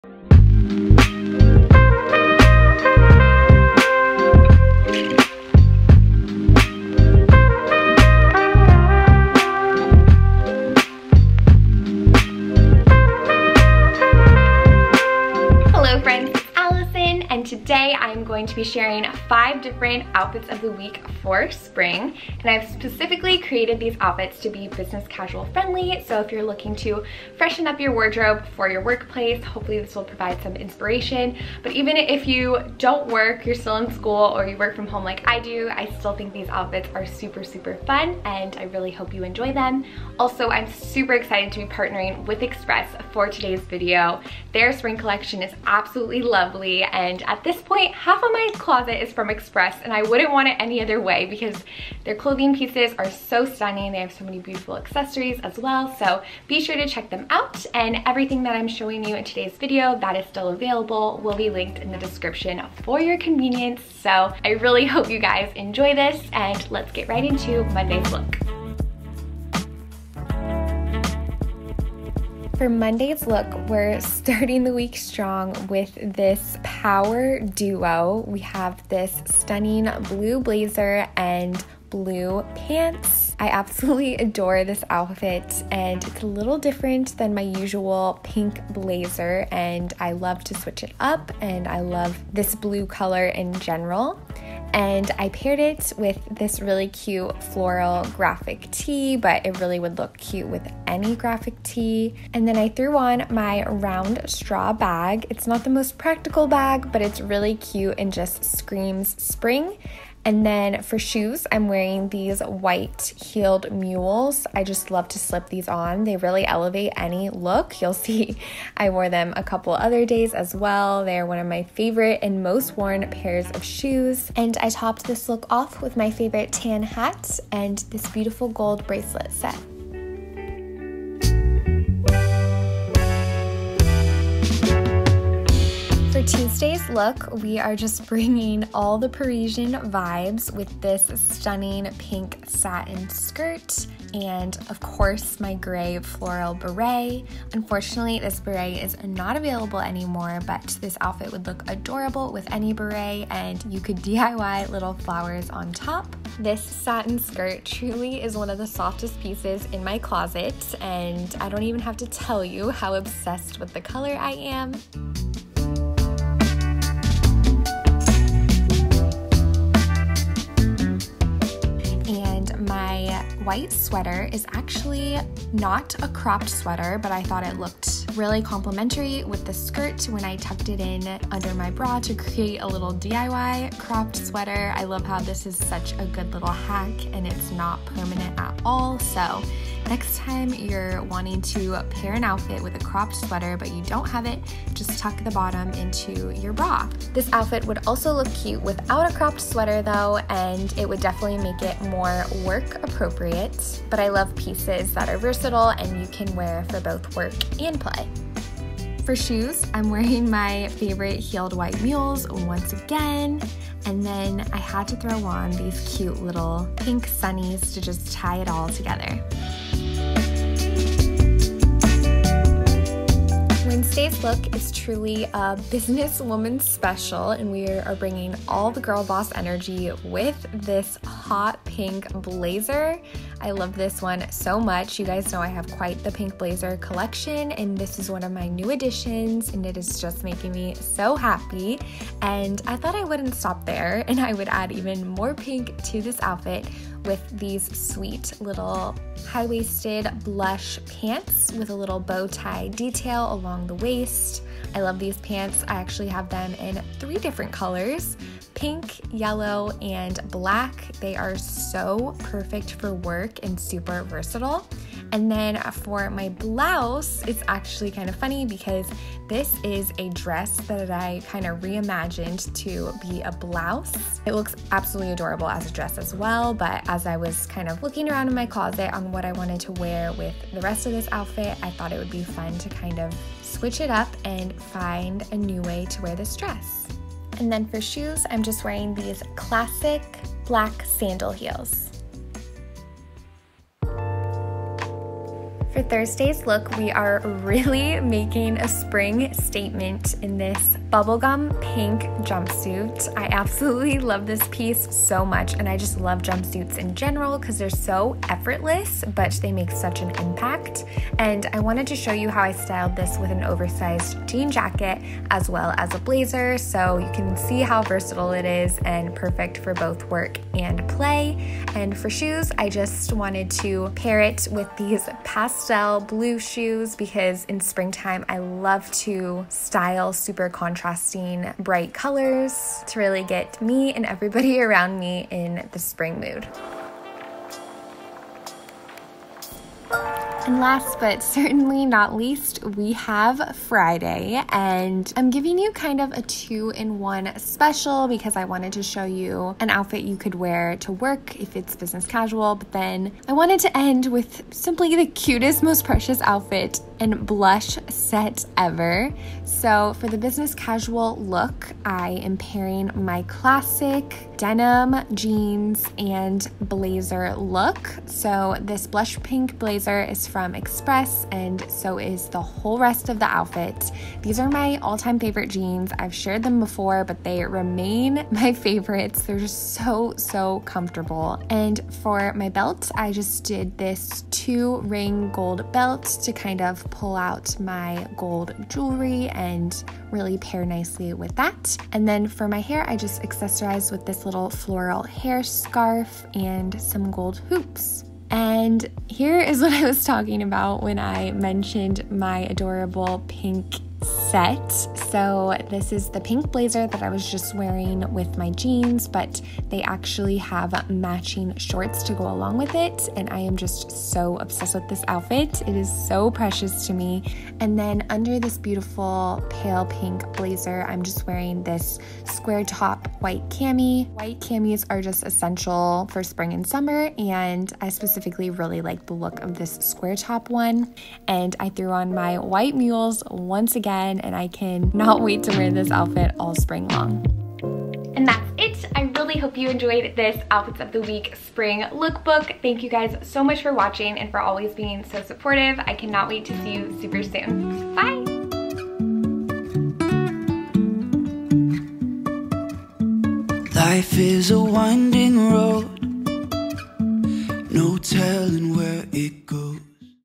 SPRING OUTFIT IDEAS | 5 outfit ideas for work or play! 🌼
Hi friends! 🌸 I hope you enjoy this spring outfits of the week video for a little business casual outfit inspiration! ✨

MONDAY:
♡ Blue blazer
♡ Blue wide-leg pants
♡ White mule shoes
♡ Gold bracelets
♡ Flower graphic tee (similar)
♡ Tan hat

TUESDAY:
♡ Pink satin skirt
♡ White sweater (similar)
♡ White mule shoes
♡ Pink sunglasses (similar)

WEDNESDAY:
♡ Blush bow ankle pants
♡ Dotted puff sleeve dress
♡ Pink blazer
♡ Black sandal heels

THURSDAY:
♡ Radiant pink jumpsuit
♡ Pink tailored blazer
♡ Oversized denim jacket (similar)

FRIDAY:
♡ Pink tailored blazer
♡ Pink tailored shorts
♡ Dark-wash ankle jeans
♡ Double ring belt
♡ Square neckline cami
♡ Hair scarf (similar)

Let's create something incredible!💡 Be truly glad...there is wonderful joy ahead. ♡ 1 Peter 1:6